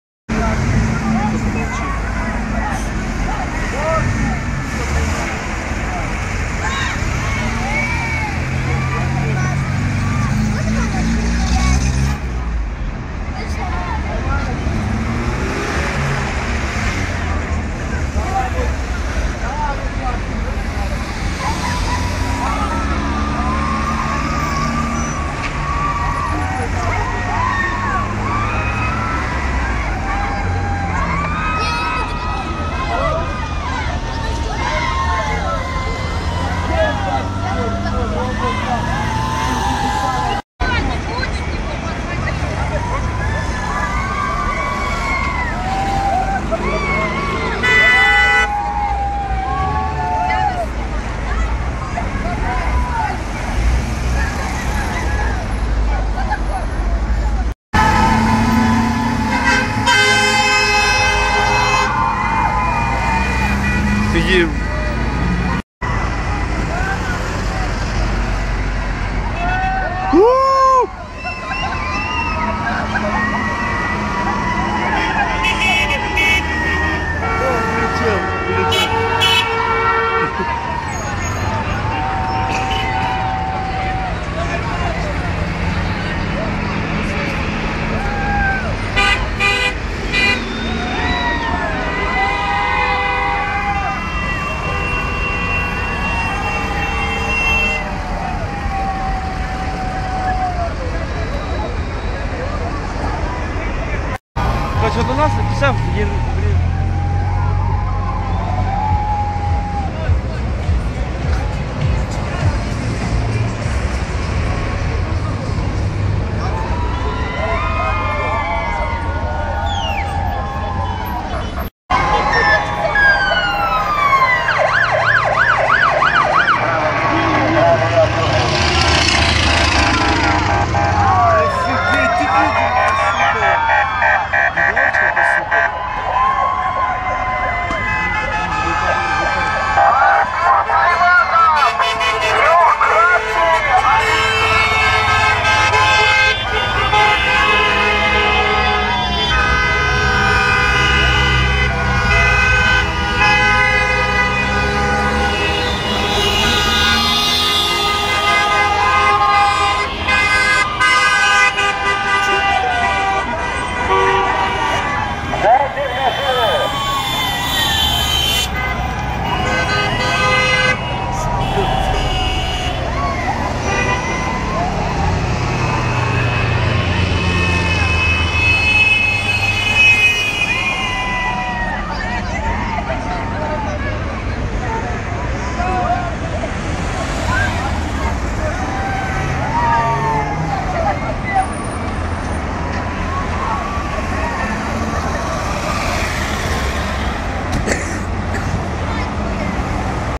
Встреча Артура Таймазова на Родине во Владикавказе (Кировский мост)
Встреча трехкратного Олимпийского чемпиона по вольной борьбе Артура Борисовича Таймазова, 2012 год